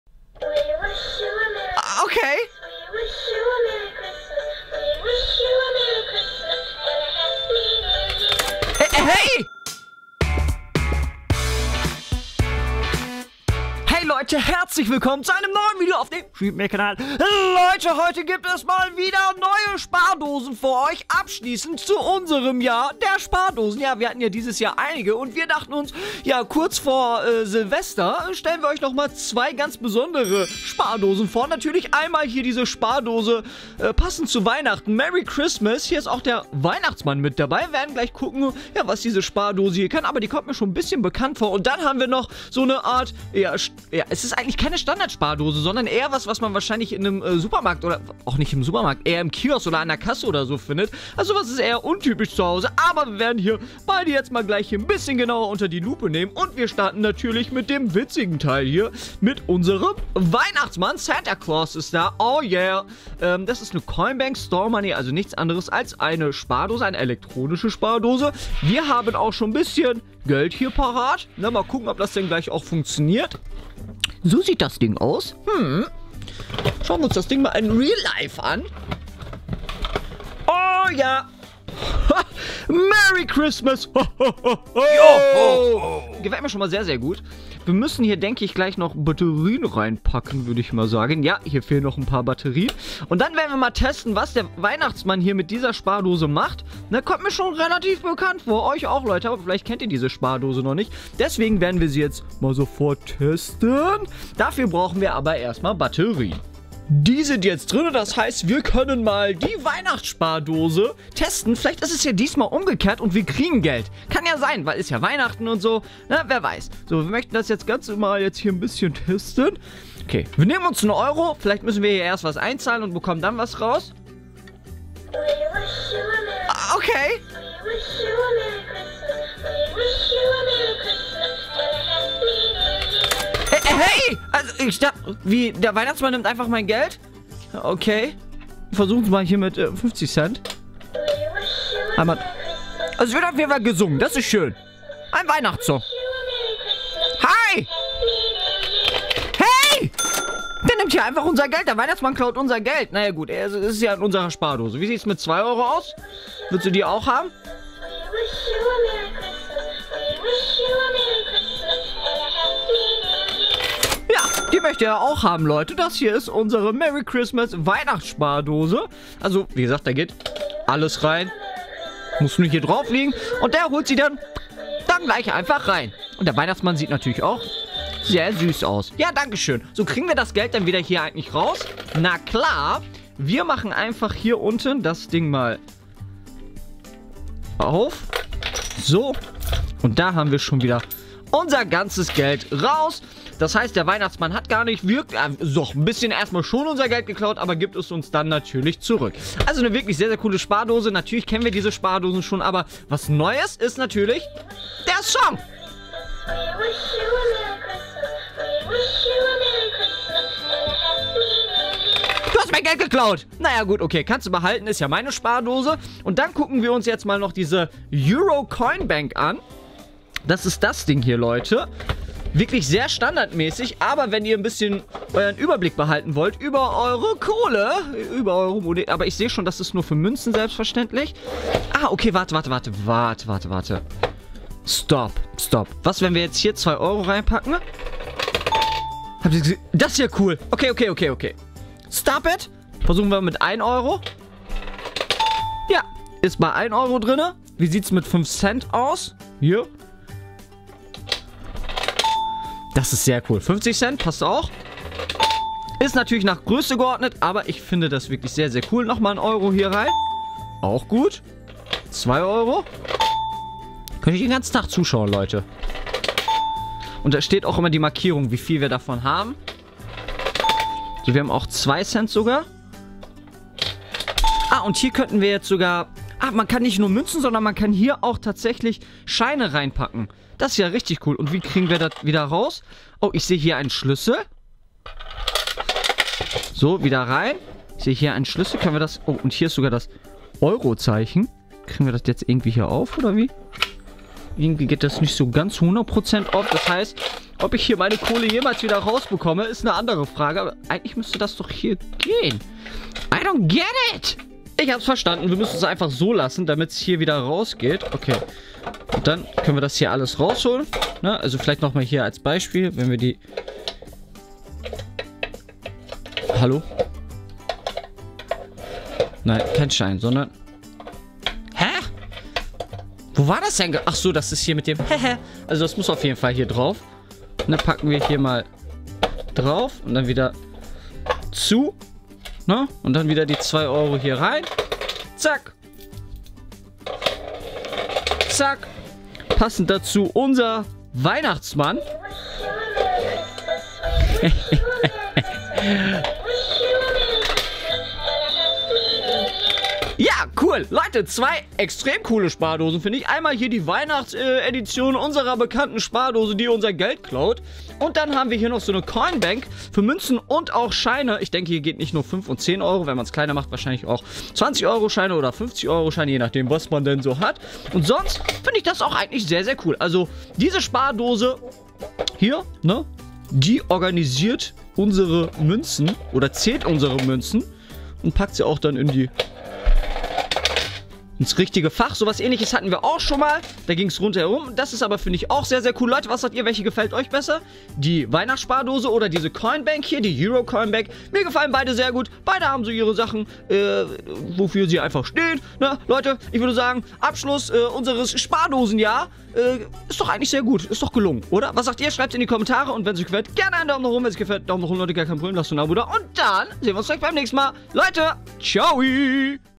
1 SPARDOSE SORTIERT DEIN GELD & 1 SPARDOSE KLAUT DEIN GELD! 2 Witzige Coin Banks für Kinder! Deutsch
Leuteeee, wir haben mal wieder zwei richtig coole Spardosen, die wir euch gerne zeigen möchten! Die eine klaut, die andere sortiert! Na, welche gefällt euch besser?

Wow! Du hast tatsächlich bis hierher gelesen! Deine EXTRA Frage: Seid ihr sparsam oder gebt ihr euer Taschengeld immer gleich aus?

16734